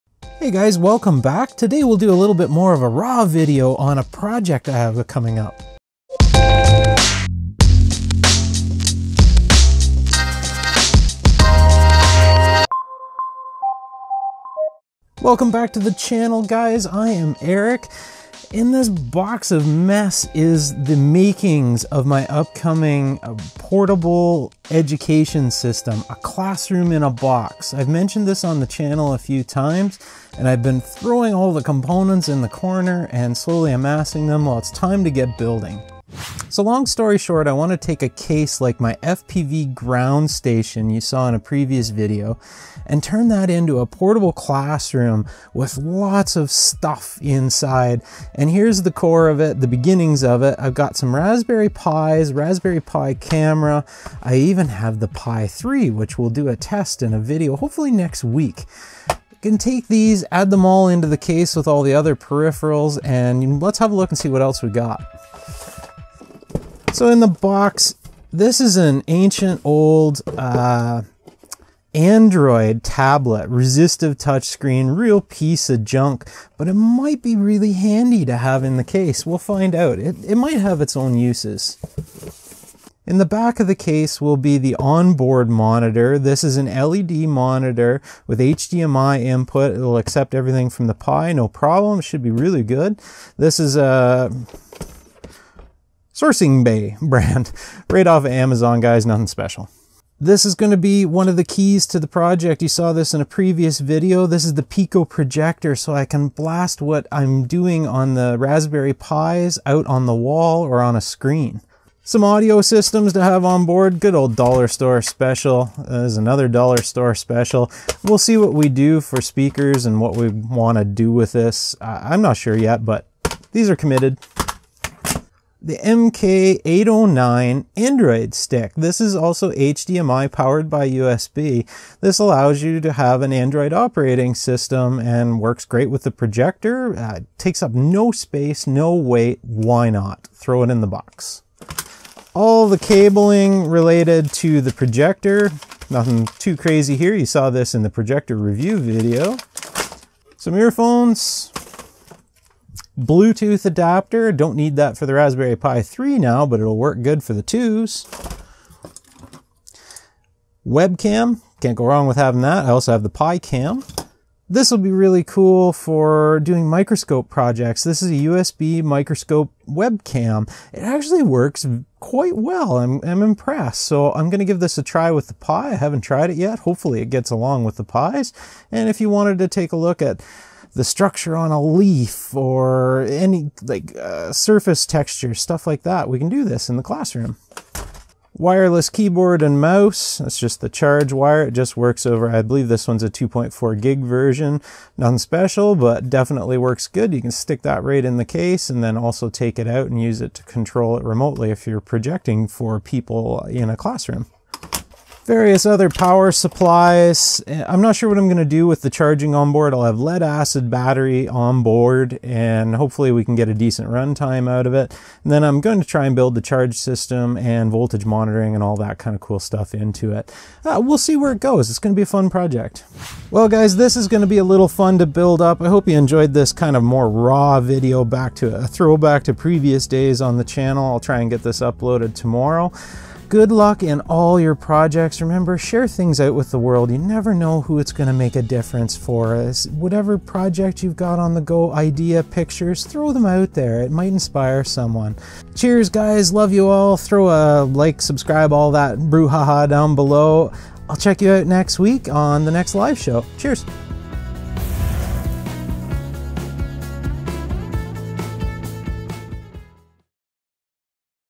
Portable Classroom With Raspberry Pi- EduCase Parts Overview
Parts and electronics for my portable education system project

This is the first look at the portable classroom in a box I want to build over the upcoming months. It will use multiple Raspberry  Pi computer boards as well as the Pico Projector system, monitor, and the Outernet Satellite receiver system I tested in previous videos.

Should be a pretty fun project and allow people to easily transport a fully self contained "classroom" which will run on internal power or be easily charged from a car, solar system or household  power.